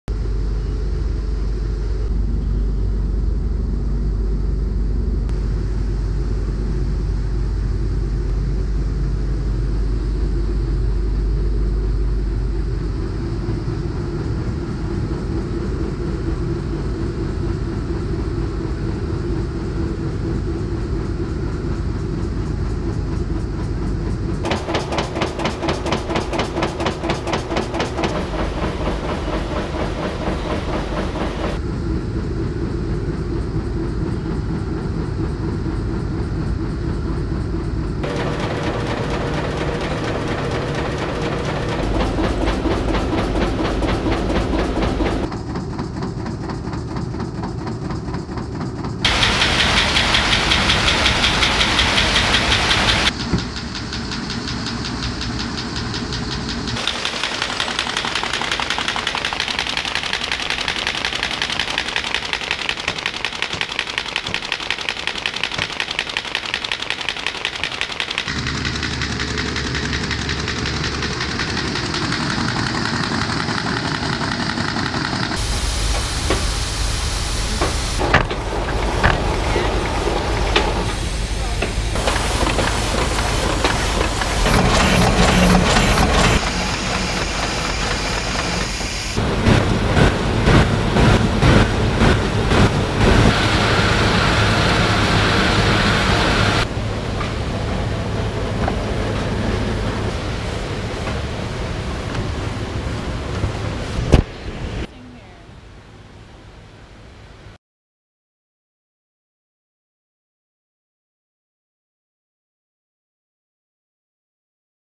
Life of a Ballot
Ever wonder how your ballot was printed, folded and shipped? Watch this video to see how your ballot was created in Maricopa County.